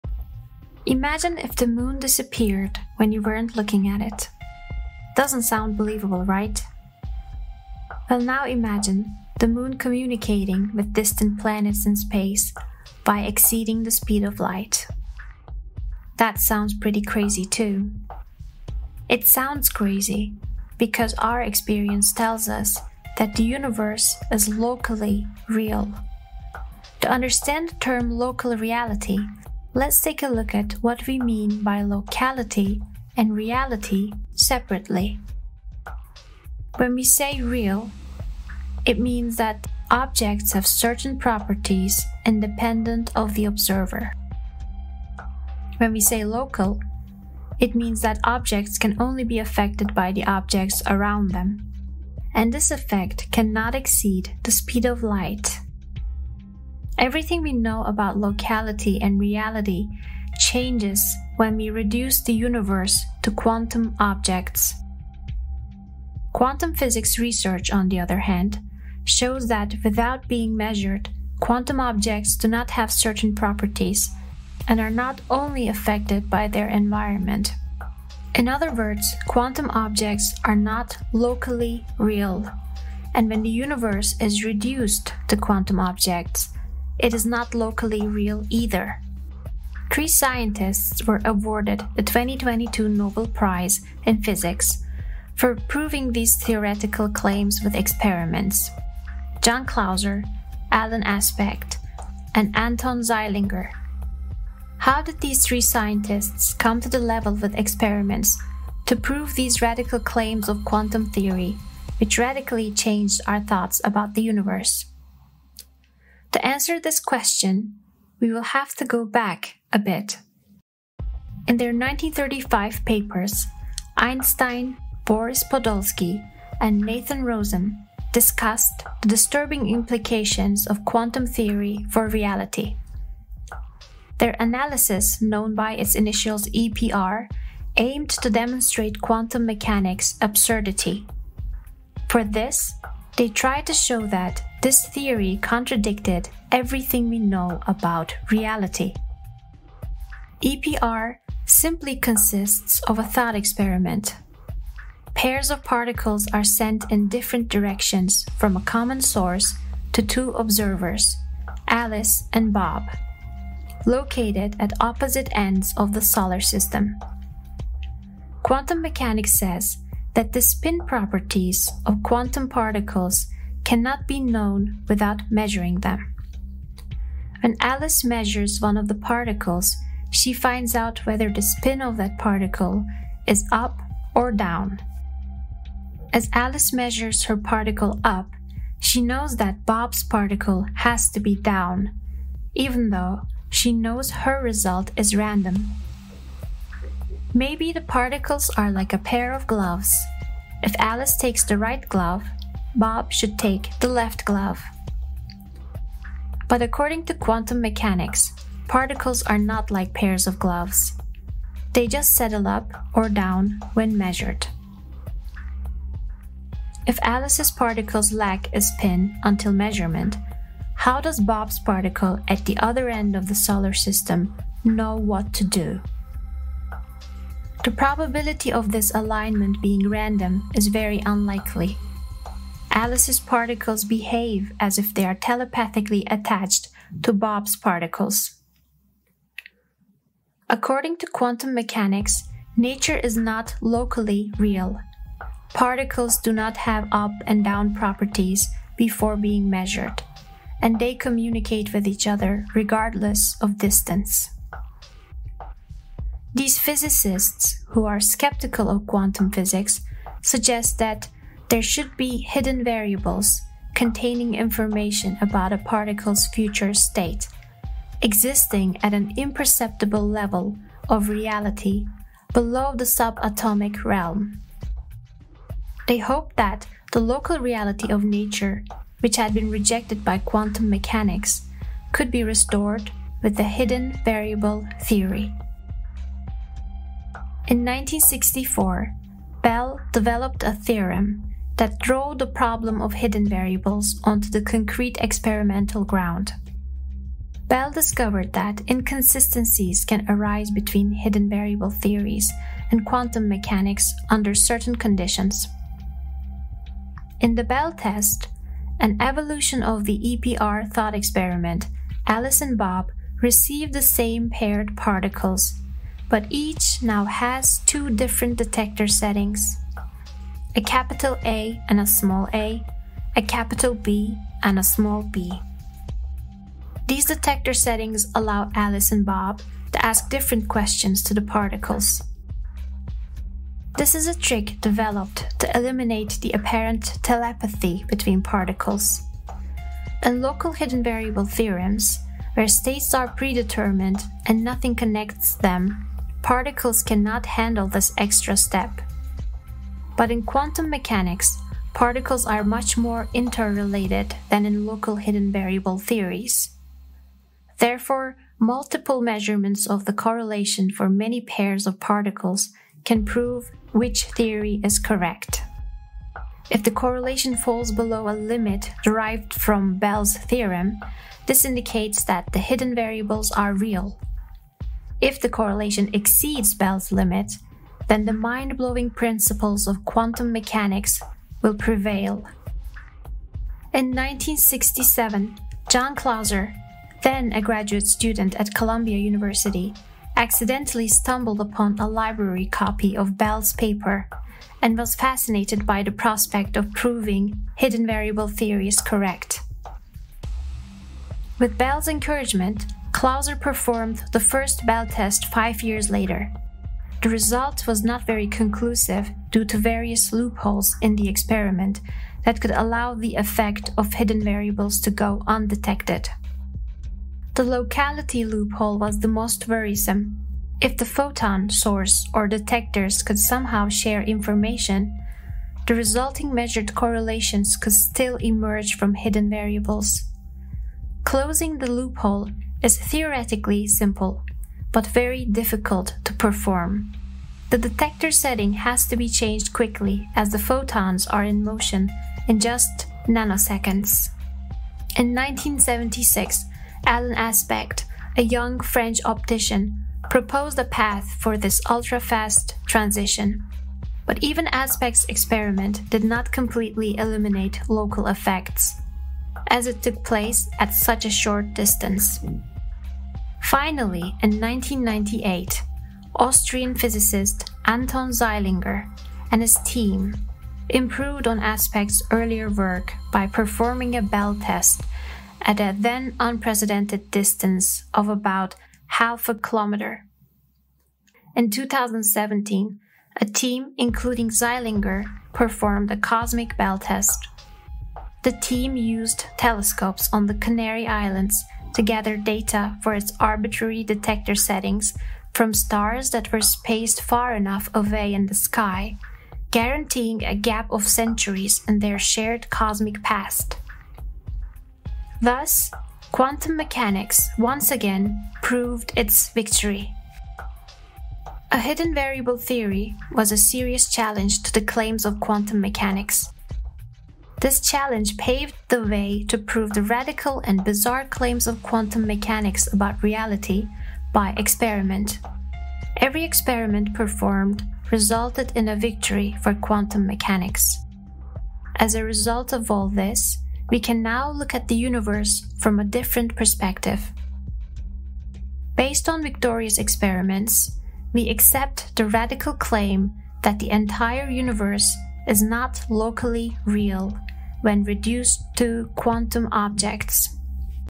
Why Is the Universe Not Locally Real? How Has It Been Proven?
Three scientists were awarded the 2022 Nobel Prize in physics for proving these theoretical claims with experiments.

John Clauser, Alain Aspect and Anton Zeilinger.

How did these three scientists come to the level with experiments to prove these radical claims of quantum theory, which radically changed our thoughts about the universe?

0:26  What does it mean to not be locally real?
1:29 What was the Nobel Prize in Physics 2022 awarded for?
1:56 What are the main arguments of the EPR paper?
4:16 What do The Hidden Variable Theories try to explain?
4:46 How does Bell's Theorem disprove Local Hidden Variable Theories?
6:23 How did John Clauser bring Bell's theorem to the experiment?
8:25 Conclusion: The Victory of Quantum Physics